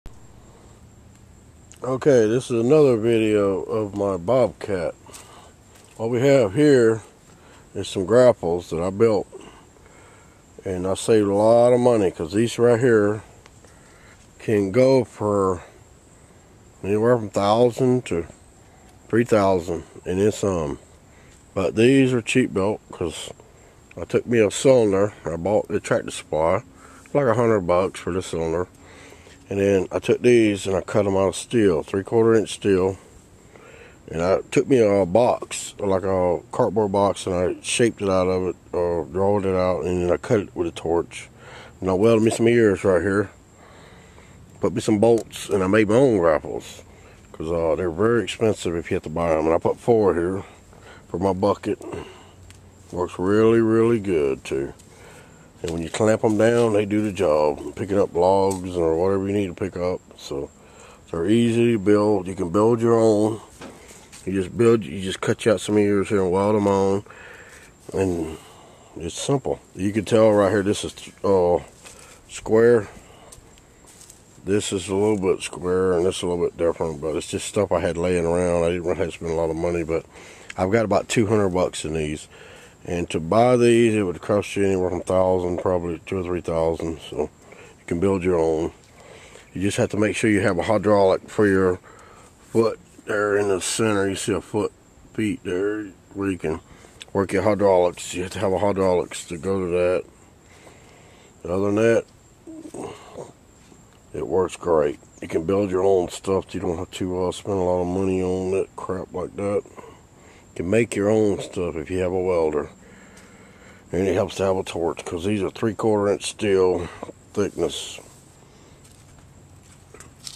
How to make a $2000 Bobcat Grapple For Only $200
how to build a 2000$ bobcat grapple for only 200$ Small engine repair. All homemade